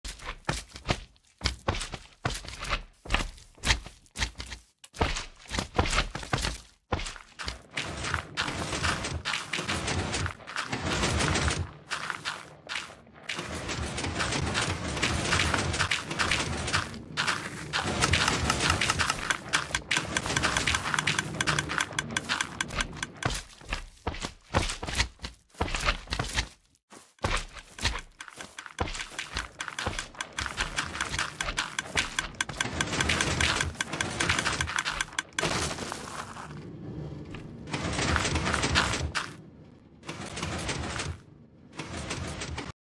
Redstone Helicopter be like...
Melon farm collects melon pieces, that are being converted by Composter to bonemeal, which makes the vine grow fast.